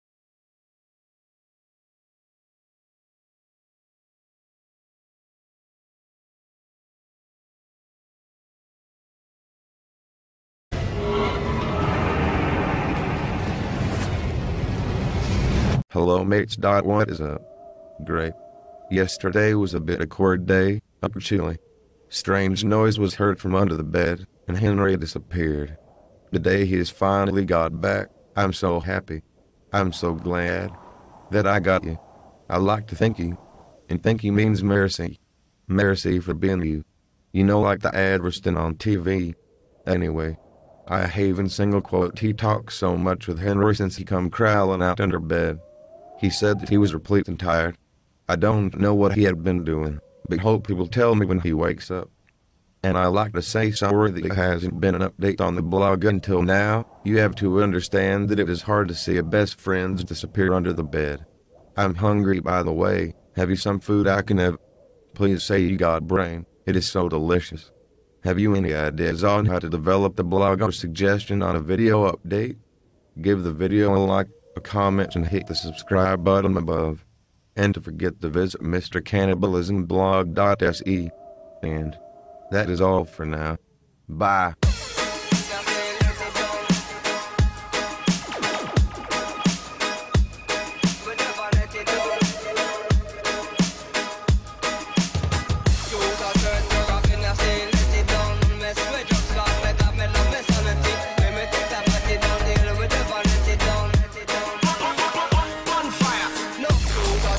Mr Cannibalism - Henry is back
Hello m8's!

Have ya heard? Henry is back! I'm still not sure what happend under the be.. He is asleep right now so I can't ask him. He said something about that he was replete, so I wonder what he had done under the bed, Hopefully we soon get to know and before that ya can like and comment the video and hit the subscribe buttom above the clip.